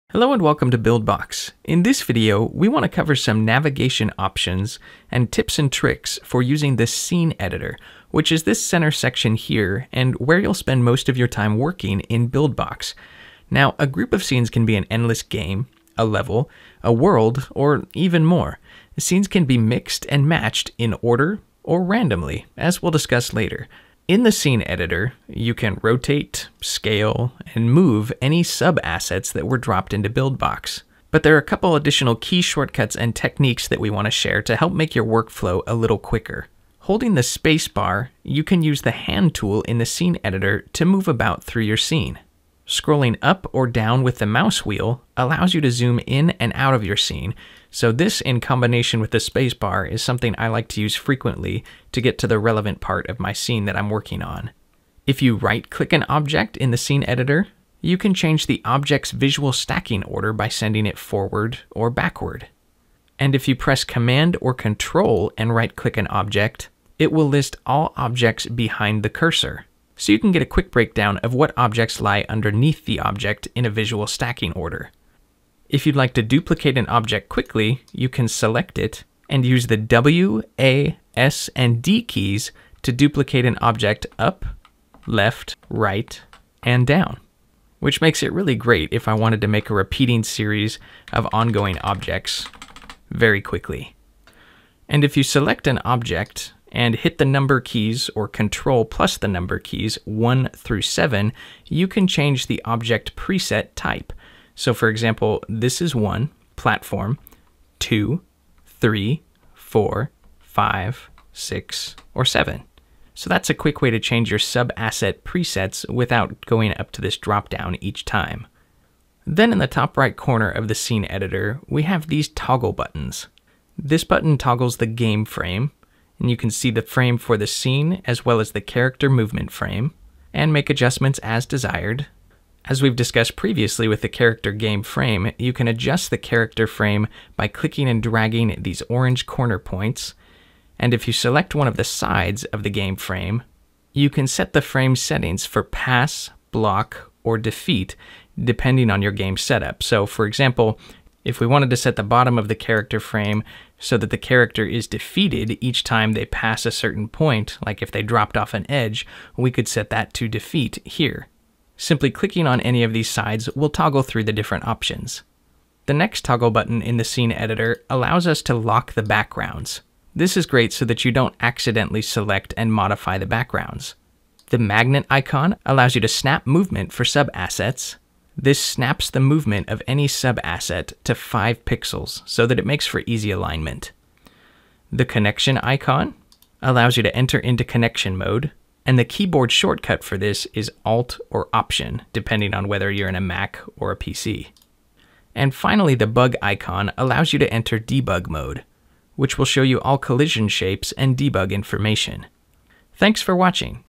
Scene Editor - Buildbox 2 Tutorial
In this Buildbox 2.0 Tutorial you'll learn how to use the navigation options and a few cool tips and tricks for using the scene editor when making your game.